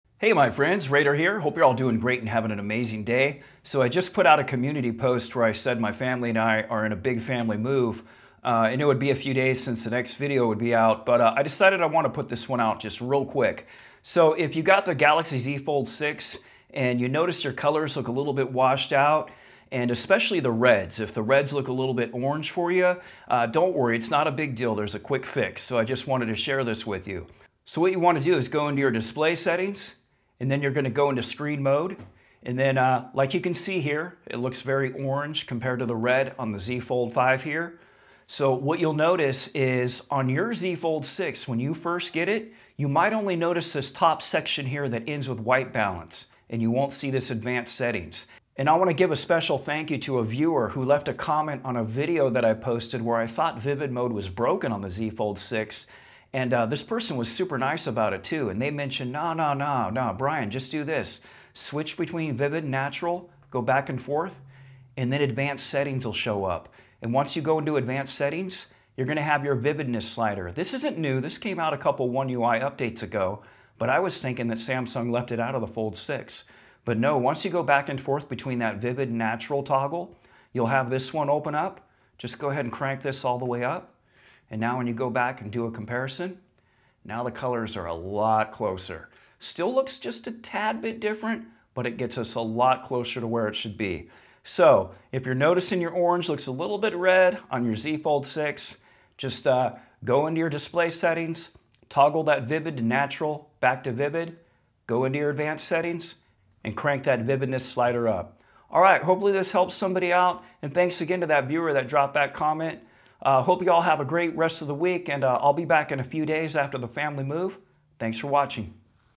Samsung Galaxy Z Fold 6 | Colors Washed Out? Red looks Orange? Let's Fix It!
If the colors on your new Samsung Galaxy Z Fold 6 look a little off, this video might do the trick for you.

A special thanks to a mystery viewer who helped me figured this out, as I thought Vivid mode was broken for a bit.

I'll be back in a few days after my family and I wrap up our move.